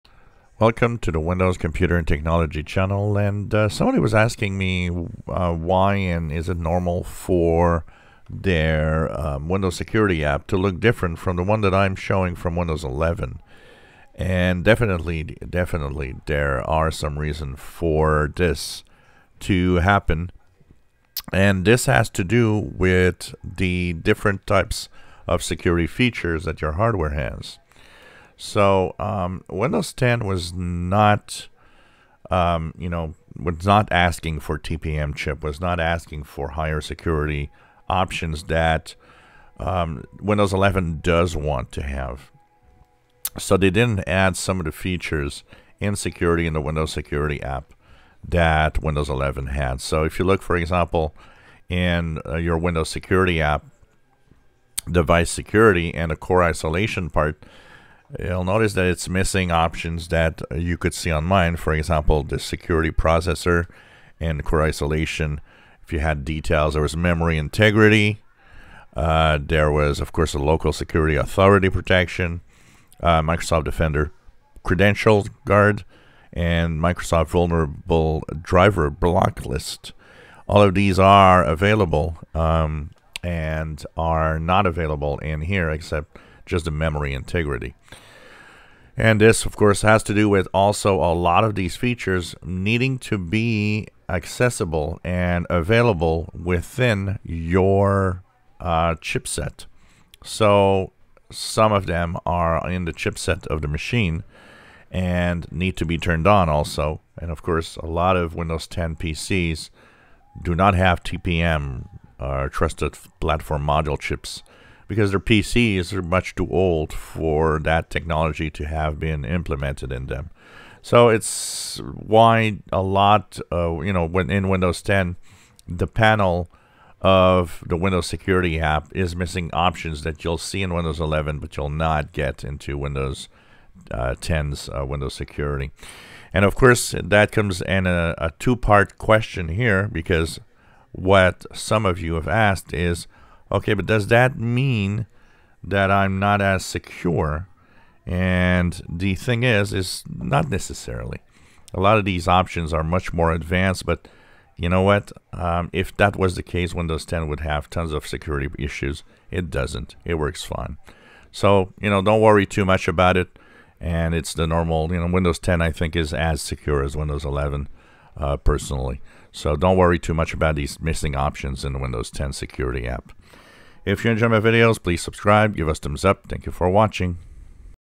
Windows 10 Security app and Windows 11 VS 10 security
Missing security options in Windows 10 or 11 Security app does not mean you are less secure